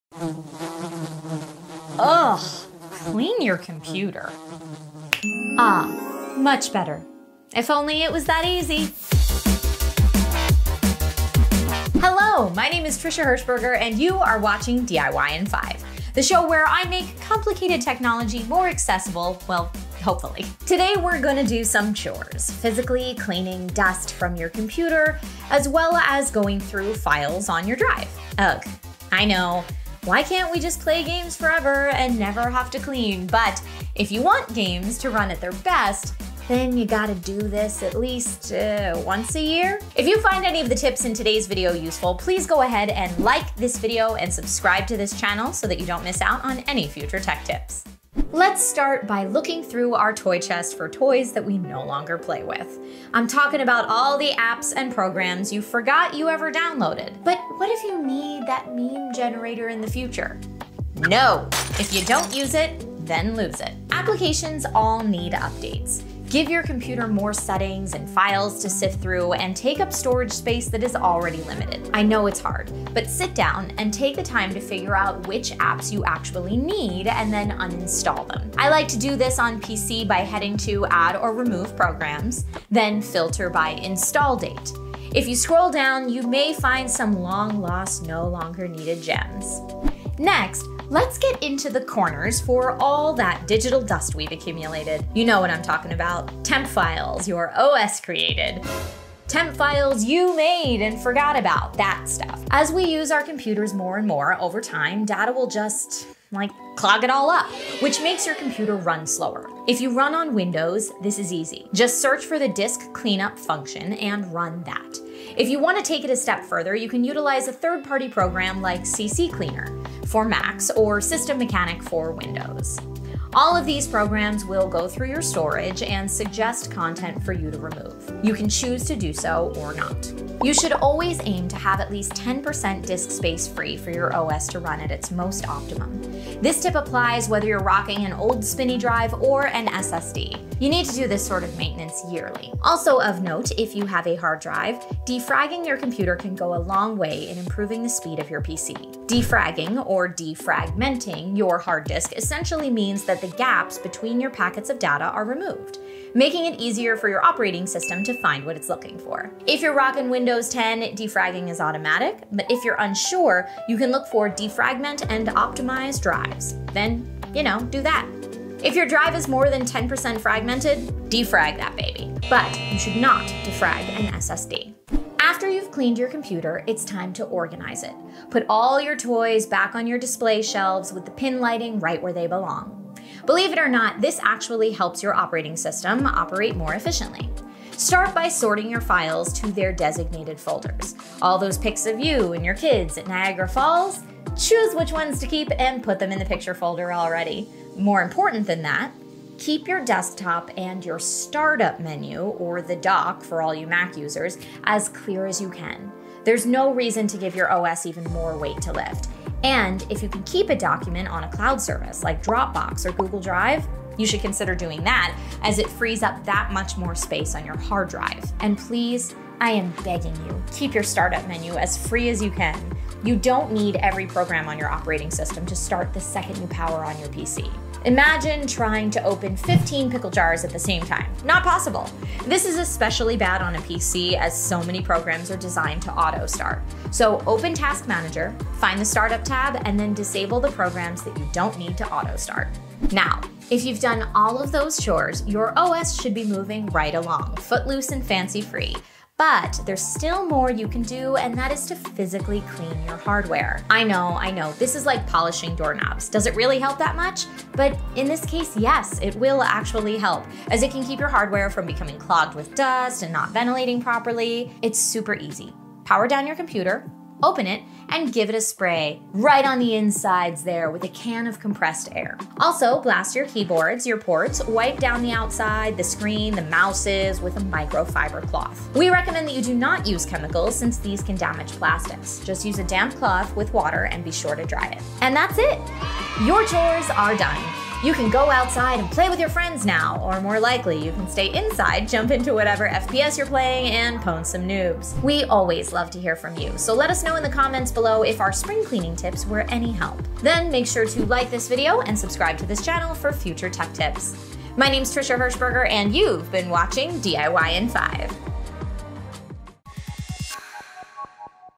PC LENTO? Aprenda a limpar seu computador e liberar espaço 🖥🚿 - Dicas para PC ep13 - Kingston Brasil
Seu computador está lento, demora pra carregar os arquivos, jogos, pastas e programas? Vem aprender a limpar o computador por dentro & por fora, liberando memória e deixando ele muito mais rápido nesse episódio do DIY em 5 com Trisha!

Você sabe como se livrar dos programas antigos de maneira rápida? Ou então como limpar sua sujeira digital? Ou talvez que organizar os arquivos do sistema ajuda o seu computador a ficar mais rápido?

Você sabia que ferramentas como CC Cleaner, System Mechanic, Dropbox ou Google Drive para ajudar a manter a saúde e a rapidez do seu computador? Vem aprender como! ;)

E por último mas não menos importante: como limpar FISICAMENTE seu equipamento?

Então vem descobrir tudo isso nesse episódio!

E se você deseja rodar seus jogos e programas da melhor maneira possível é recomendado que você faça essa faxina pelo menos uma vez por ano! ;)

Conta pra gente nos comentários, quando foi a última vez que você limpou seu computador?